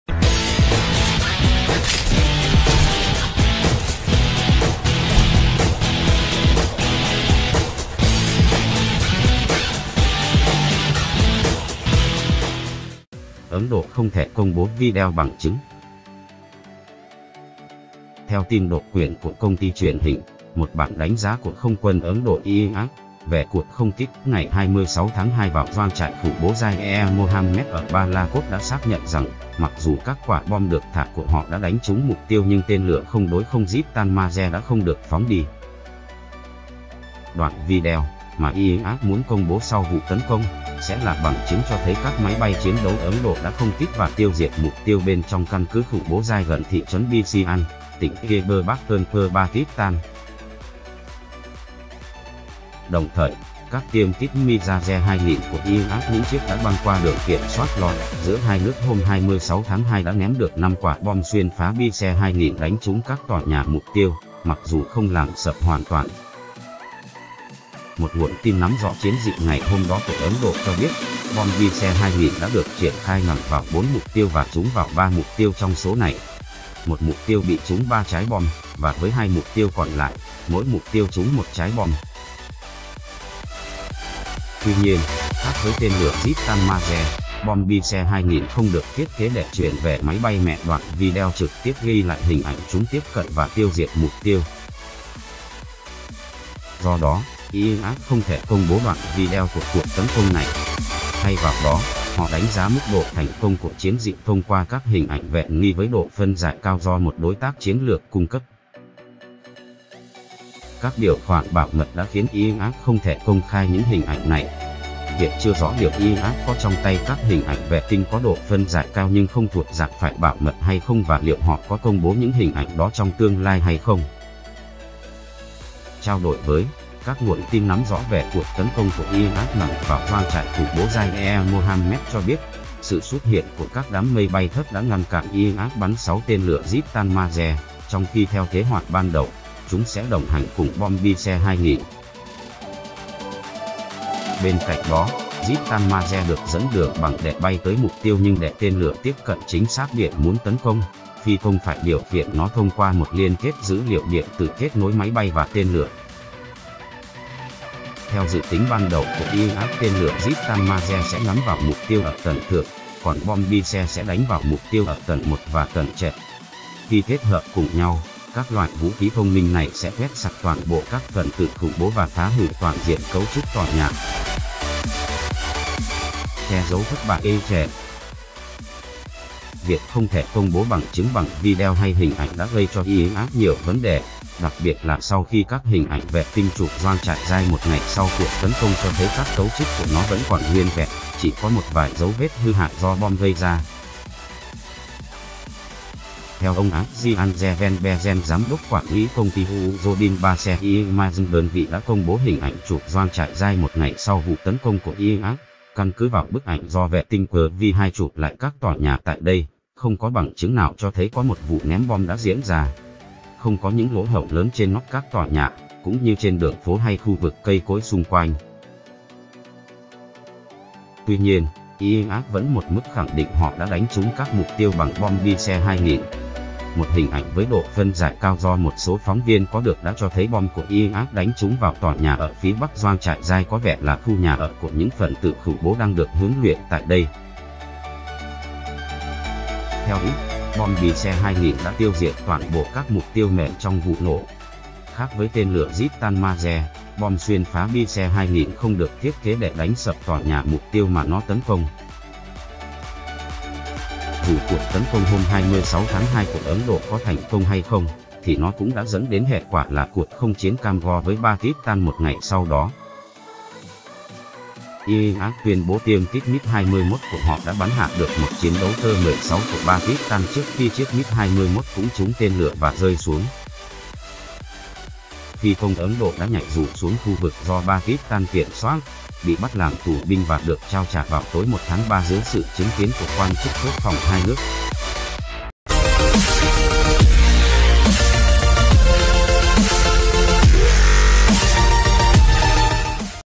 NDTV đưa tin độc quyền: Vì sao Ấn Độ không đưa ra nổi bằng chứng về vụ không kích vào Pakistan?
Ấn Độ không thể công bố video bằng chứng
Theo tin độc quyền của công ty truyền hình NDTV, một bản đánh giá của Không quân Ấn Độ (IAF) về cuộc không kích ngày 26/2 vào doanh trại khủng bố Jaish-e-Mohammed ở Balakot đã xác nhận rằng, mặc dù các quả bom được thả của họ đã đánh trúng mục tiêu nhưng tên lửa không-đối-không Crystal Maze đã không được phóng đi.

Đoạn video, mà IAF muốn công bố sau vụ tấn công, sẽ là bằng chứng cho thấy các máy bay chiến đấu Ấn Độ đã không kích và tiêu diệt mục tiêu bên trong căn cứ khủng bố Jaish gần thị trấn Bisian, tỉnh Khyber Pakhtunwa (Pakistan).
Đồng thời, các tiêm kích Mirage 2000 của IAF – những chiếc đã băng qua Đường kiểm soát (LoC) giữa hai nước hôm 26/2 – đã ném được 5 quả bom xuyên phá SPICE 2000 đánh trúng các tòa nhà mục tiêu, mặc dù không làm sập hoàn toàn.
Một nguồn tin nắm rõ chiến dịch ngày hôm đó của Ấn Độ cho biết, bom SPICE 2000 đã được triển khai nhằm vào "4 mục tiêu" và "trúng vào 3 mục tiêu trong số này". Một mục tiêu bị trúng 3 trái bom, và với hai mục tiêu còn lại, mỗi mục tiêu trúng 1 trái bom.
Tuy nhiên, khác với tên lửa Crystal Maze, bom SPICE 2000 không được thiết kế để truyền về máy bay mẹ đoạn video trực tiếp ghi lại hình ảnh chúng tiếp cận và tiêu diệt mục tiêu.
Do đó, IAF không thể công bố đoạn video của cuộc tấn công này. Thay vào đó, họ đánh giá mức độ thành công của chiến dịch thông qua các hình ảnh vệ tnih với độ phân giải cao do một đối tác chiến lược cung cấp.
Các điều khoản bảo mật đã khiến IAF không thể công khai những hình ảnh này. Hiện chưa rõ liệu IAF có trong tay các hình ảnh vệ tinh có độ phân giải cao nhưng không thuộc dạng phải bảo mật hay không và liệu họ có công bố những hình ảnh đó trong tương lai hay không.
Trao đổi với NDTV, các nguồn tin nắm rõ về cuộc tấn công của IAF nhằm vào doanh trại khủng bố Jaish-e-Mohammed cho biết, sự xuất hiện của các đám mây bay thấp đã ngăn cản IAF bắn 6 tên lửa Crystal Maze, trong khi theo kế hoạch ban đầu, chúng sẽ đồng hành cùng bom SPICE 2000.
Bên cạnh đó, "Crystal Maze được dẫn đường bằng GPS để bay tới mục tiêu nhưng để tên lửa tiếp cận chính xác điểm muốn tấn công, phi công phải điều khiển nó thông qua một liên kết dữ liệu điện tử kết nối máy bay và tên lửa".
Theo dự tính ban đầu của IAF "Tên lửa Crystal Maze sẽ nhắm vào mục tiêu ở tầng thượng, còn bom SPICE sẽ đánh vào mục tiêu ở tầng 1 và tầng trệt". Khi kết hợp cùng nhau, các loại vũ khí thông minh này sẽ quét sạch toàn bộ các phần tử khủng bố và phá hủy toàn diện cấu trúc tòa nhà.
Che giấu thất bại ê chề?
Việc không thể công bố bằng chứng bằng video hay hình ảnh đã gây cho IAF nhiều vấn đề, đặc biệt là sau khi các hình ảnh vệ tinh chụp doanh trại Jaish một ngày sau cuộc tấn công cho thấy các cấu trúc của nó vẫn còn nguyên vẹn, chỉ có một vài dấu vết hư hại do bom gây ra.
Theo ông Adrian Zevenbergen – Giám đốc quản lý công ty European Space Imaging (đơn vị đã công bố hình ảnh chụp doanh trại Jaish một ngày sau vụ tấn công của IAF), "Căn cứ vào bức ảnh